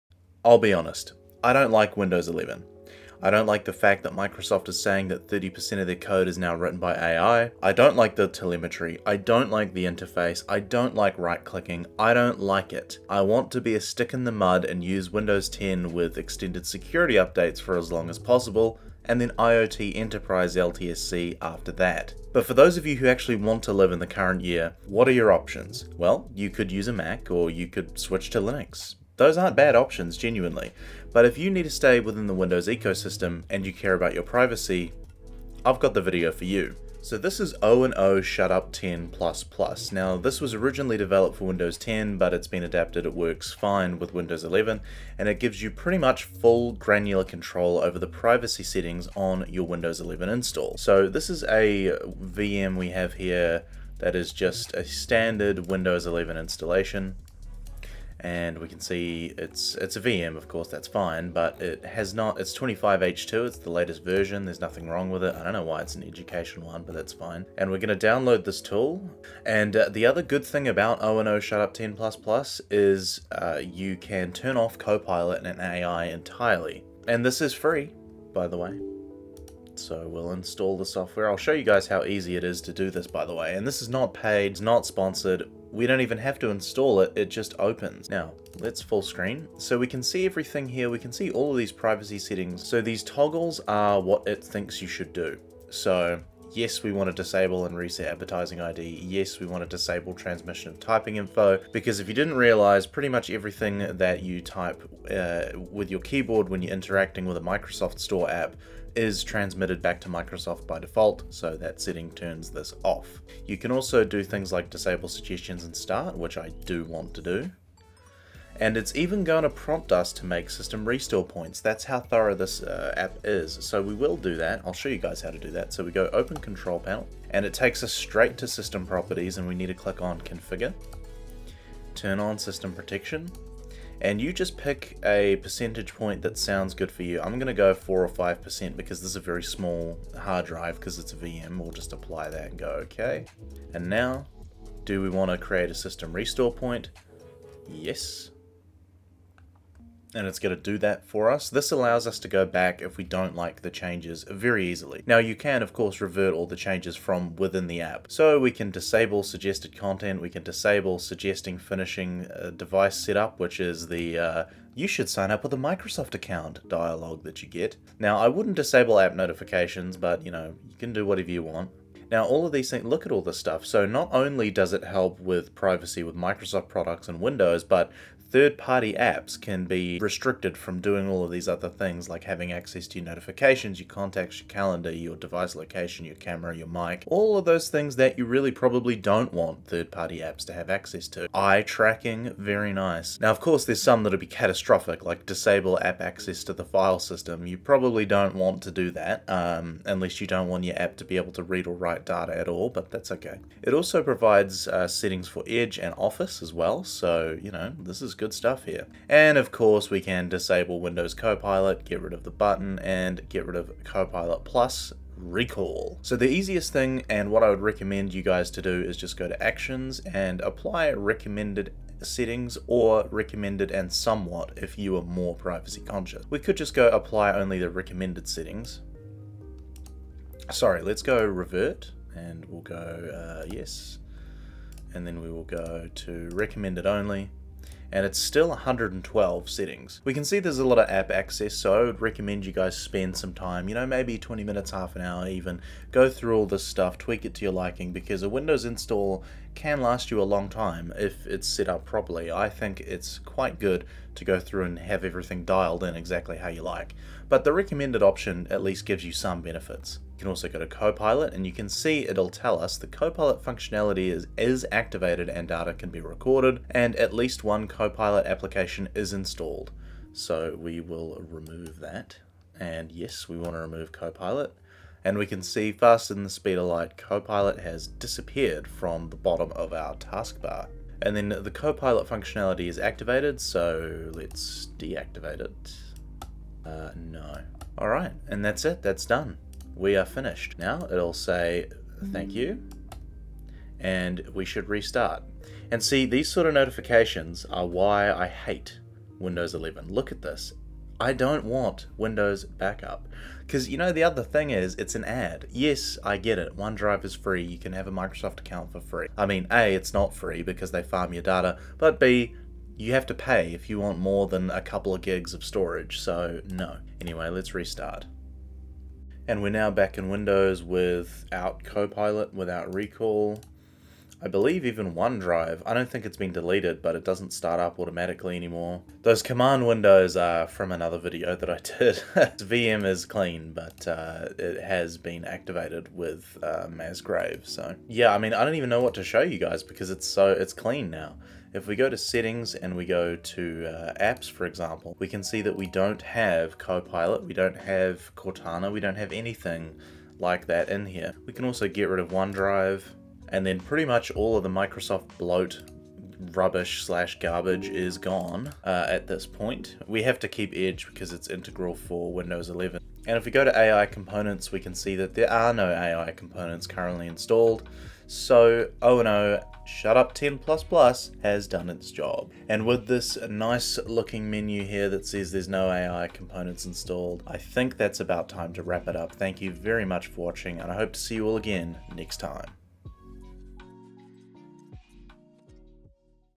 DELETE Copilot and STOP Windows SPYING in 2025 [for FREE!]
Remove AI slop and Microsoft harvesting all your data for free!

In this video we go over O&O ShutUp10++, a great piece of software you can use to stop Windows 11, Office, and even 3rd party apps sending all your data to Microsoft.